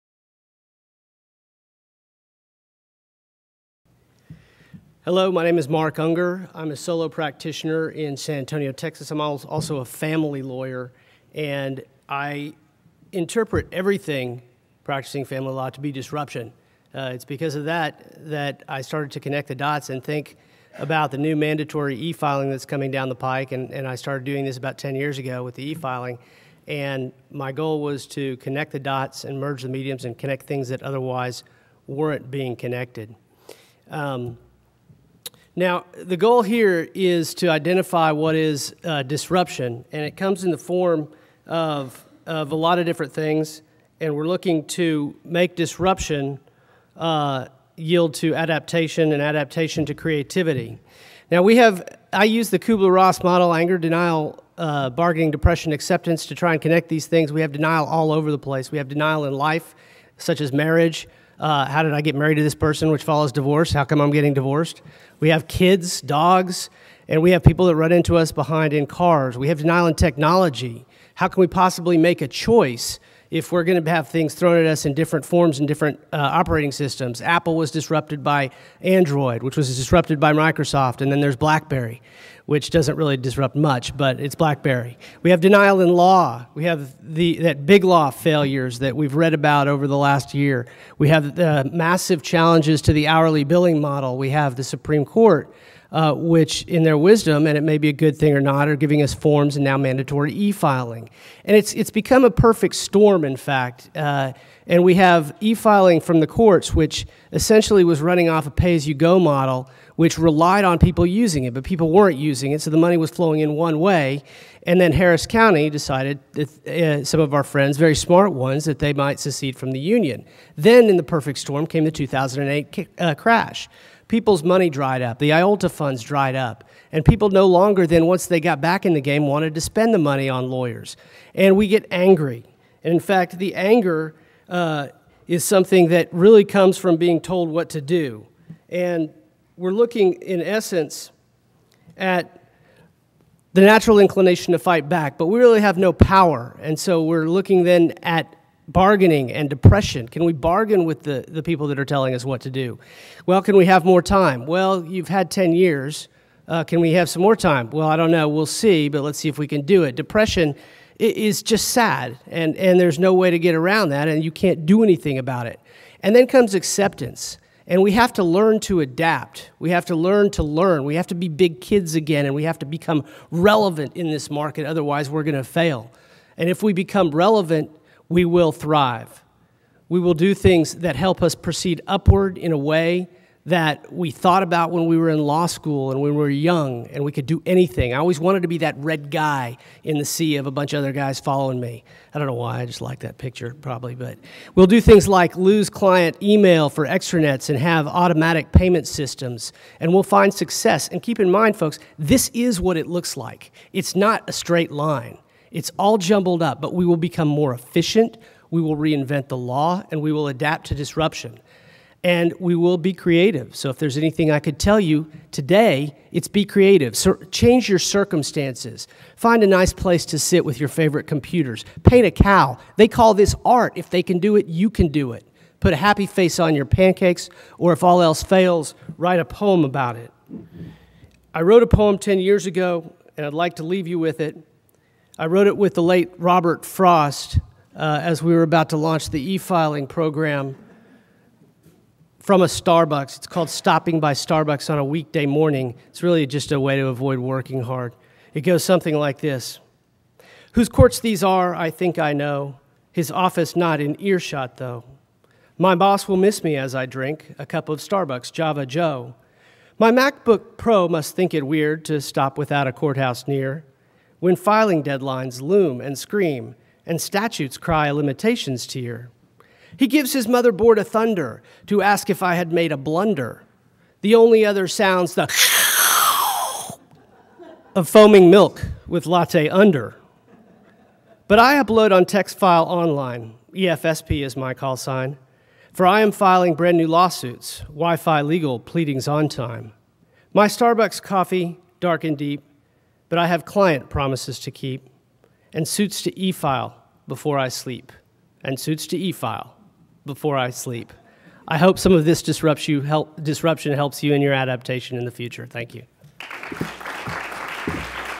Mark Unger - Ignite SBOT 2013
Mandatory e-Filing and the Five Stages of Grief: How Disruption and Adaptation Will Cost You More But Could Save Your Legal Life